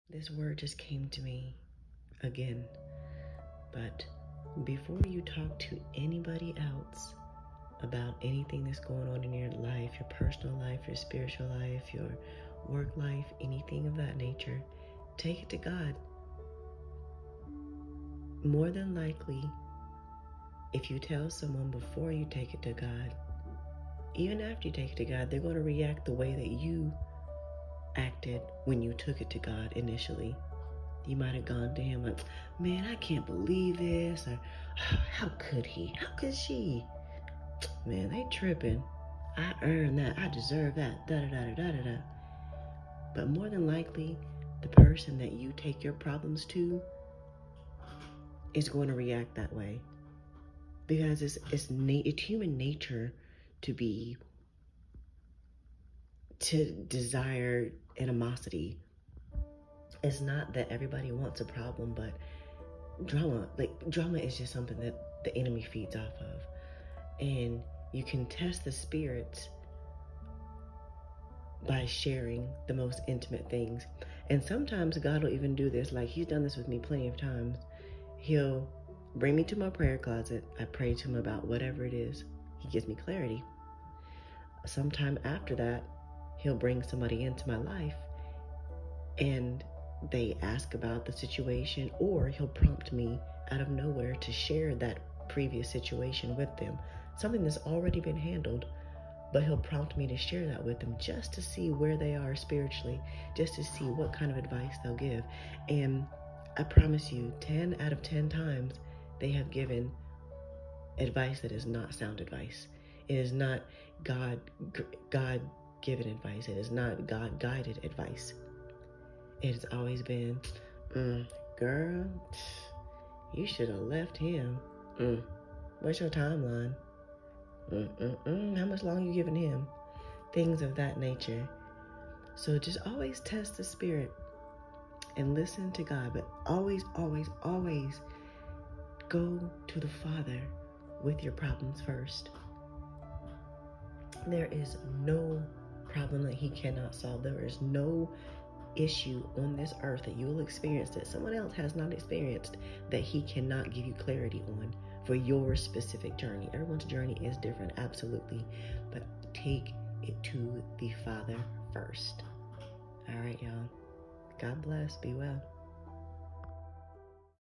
Take your issues to God FIRST!
We all have those friends and/or family members who love a good cup of tea. But do you know what they are doing with the information you share?
There are monitoring spirits, spirits of destruction, spirits of distraction and so on. Stay awake, Good People! Even your closest “buddy” could be lurking in the shadows. Not trying to put fear in anyone’s morning tea, but I am definitely sharing what God has shared with me over the past several years.

Ephesians 6:12 (GNV)
“For we wrestle not against flesh and blood, but against principalities, against powers, and against the worldly governors, the princes of the darkness of this world, against spiritual wickedness, which are in the high places.”